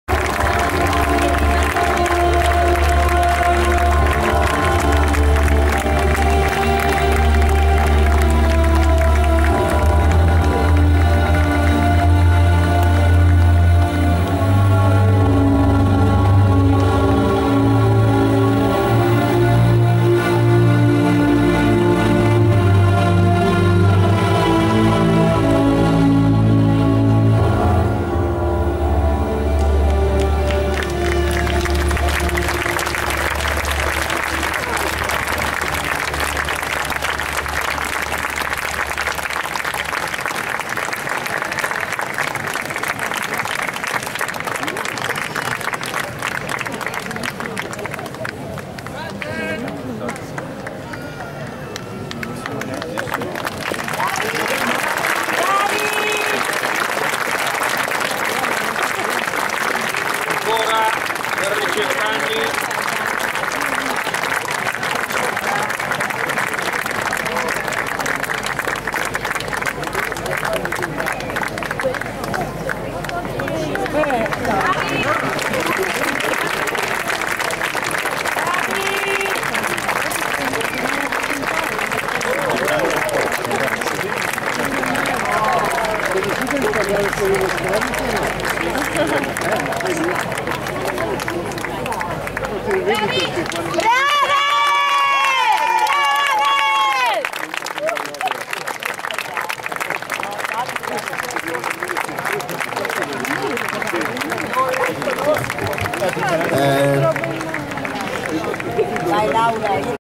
Finale Palio di Castiglione 2012
Palio di Castiglione Olona 8 luglio 2012 battaglia Milanesi contro Veneziani